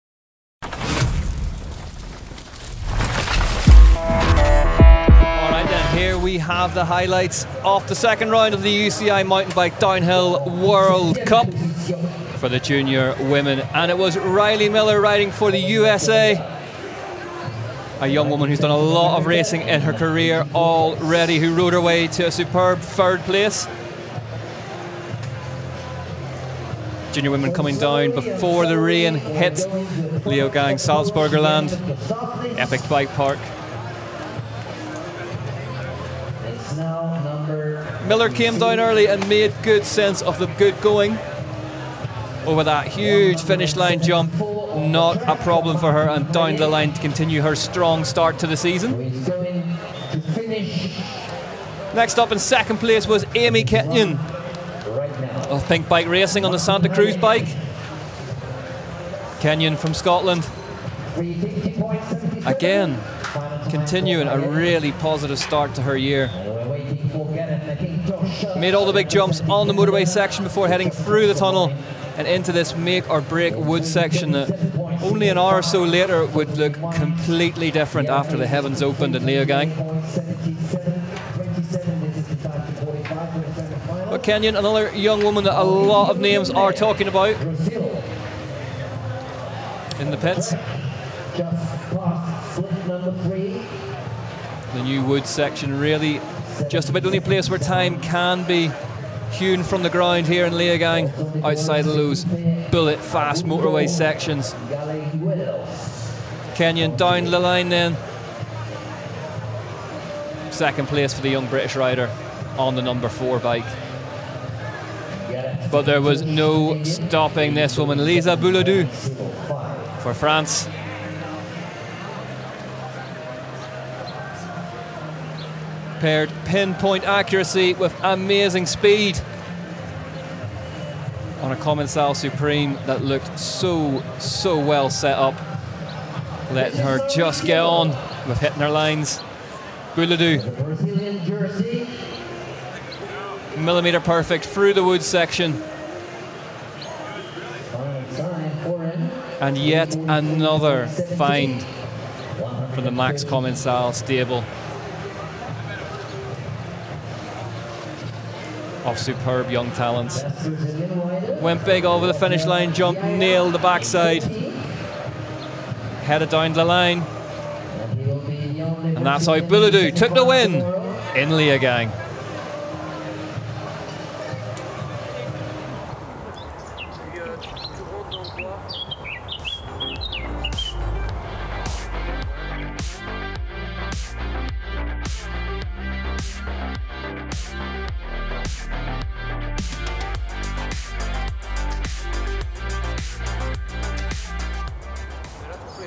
Leogang Junior Women's Downhill Finals | DHI Highlights
Enjoy these highlights from the Junior Women's Downhill finals! We'd like to apologise for the technical issue at the start of our live stream on YouTube. We were quick to raise this with the relay team who were able to reset the stream for us and get us that important audio!